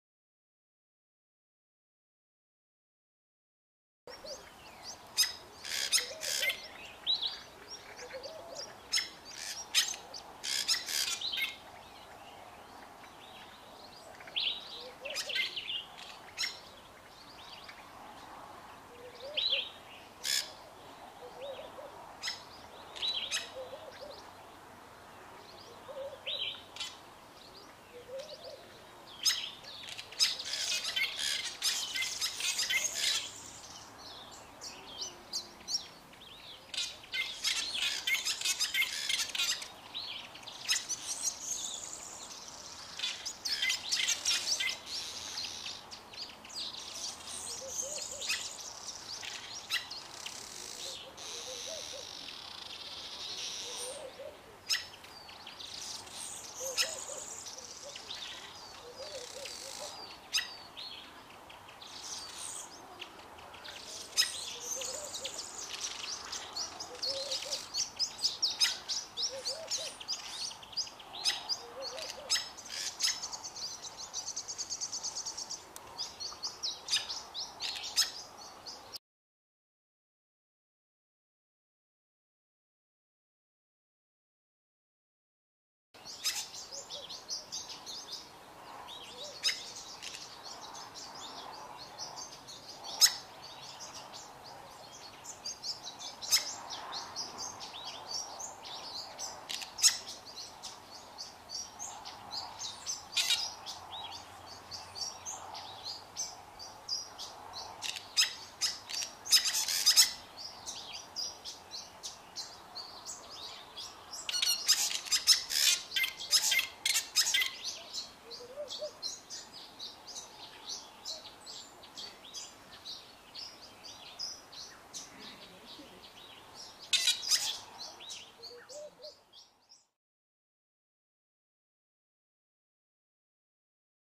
Fork-tailed Drongos singing
Watch this pair of Fork-tailed Drongos singing together.